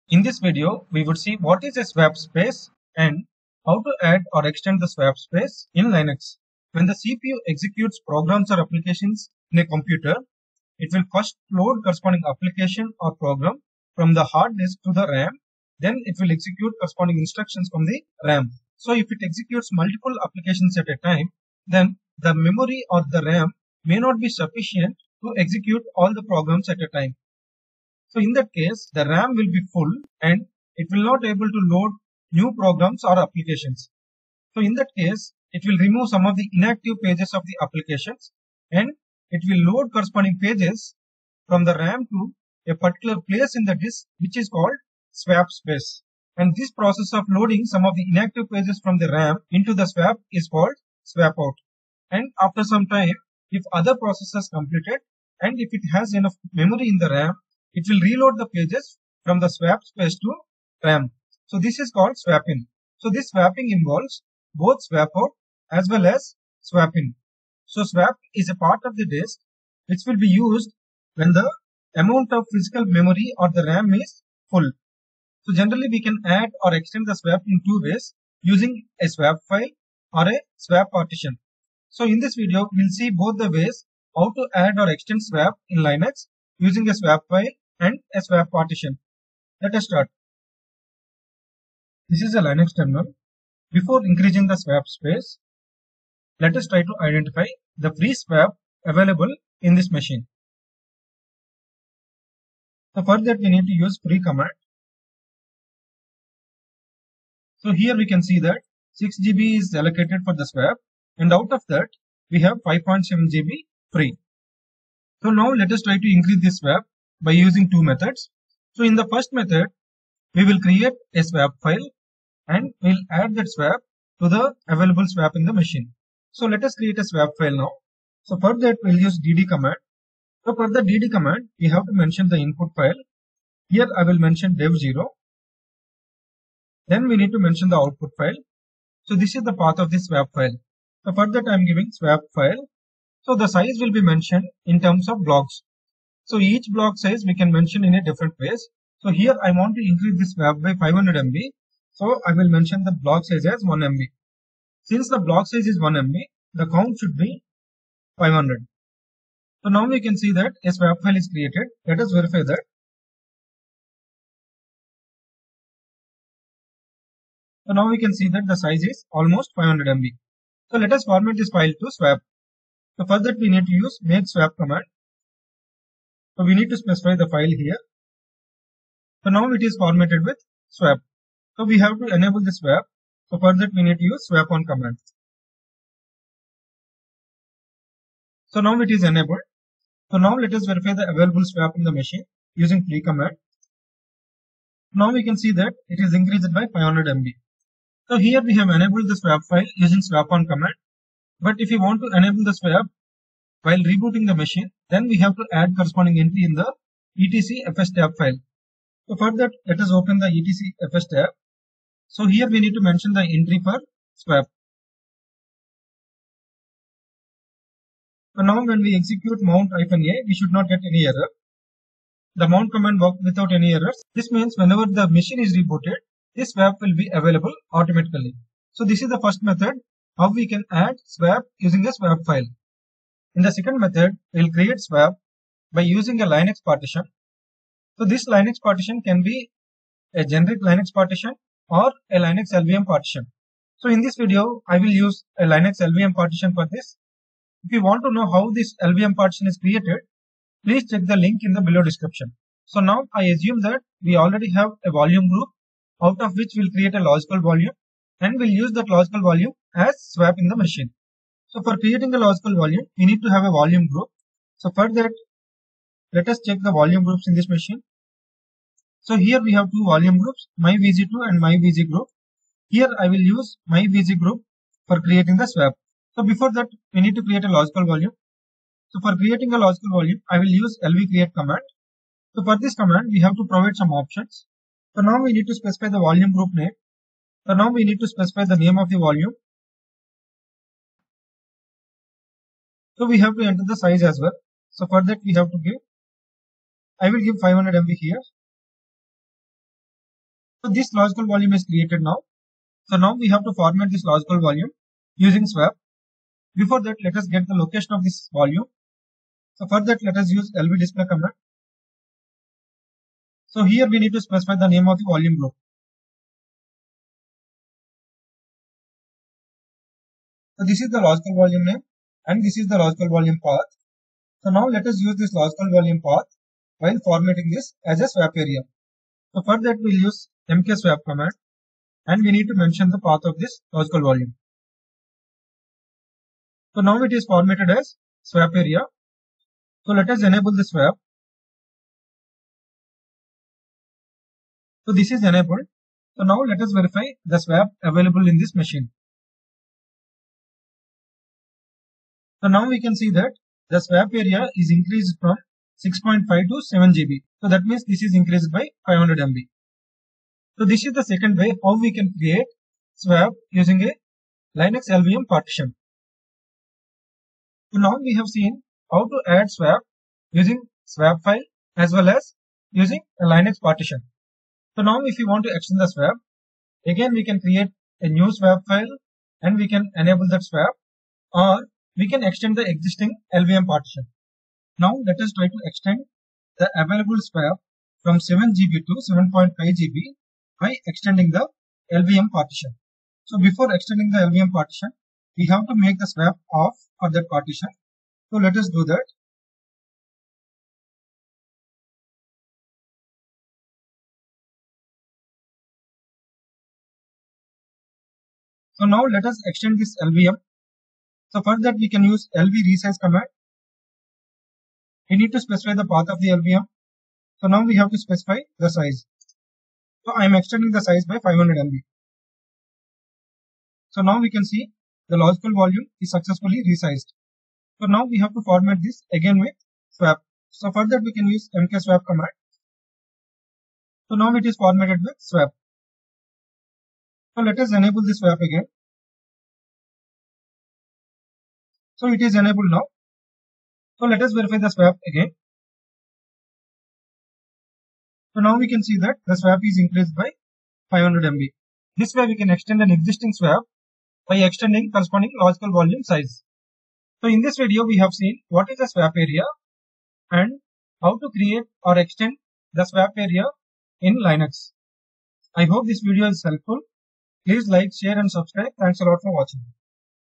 Create or Extend Swap in Linux
This video describes the below.
What is Swap Space?
How to add/extend Swap space in Linux?

For adding/extending swap, both swap file and swap partition are used in this video.